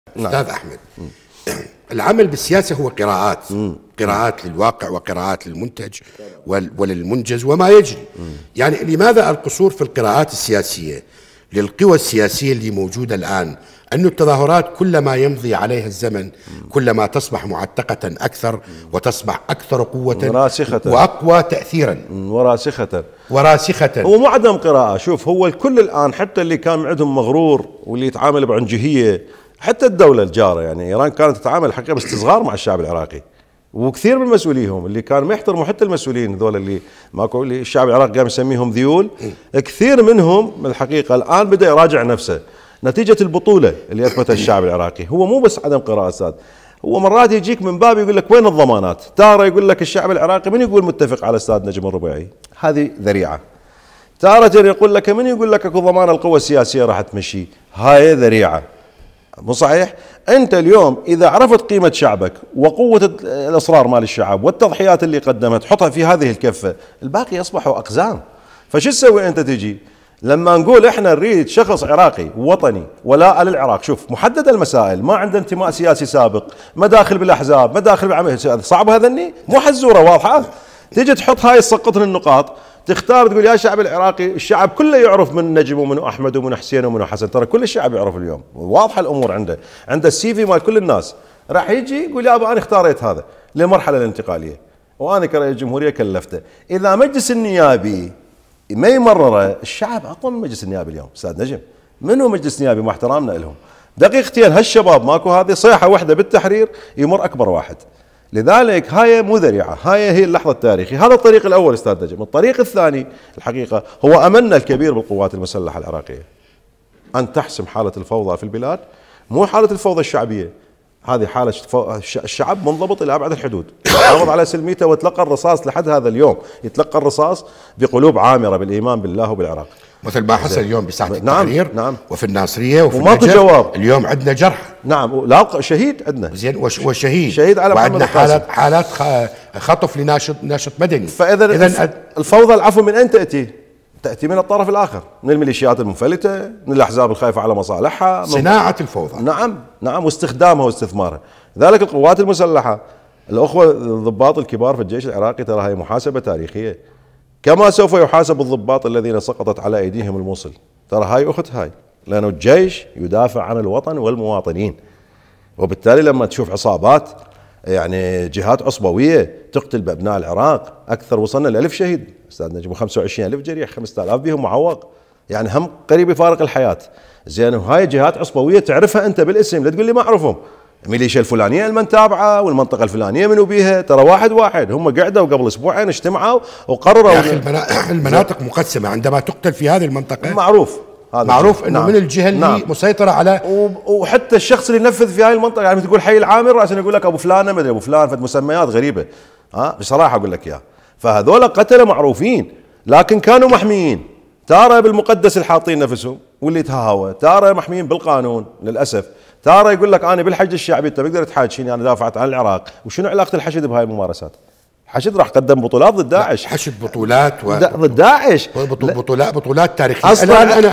أحمد الأبيض: الكثير من الذيول بدأوا بمراجعة أنفسهم نتيجة البطولات التي قدمها الشعب العراقي
لقاء نجم_الربيعي مع المحلل السياسي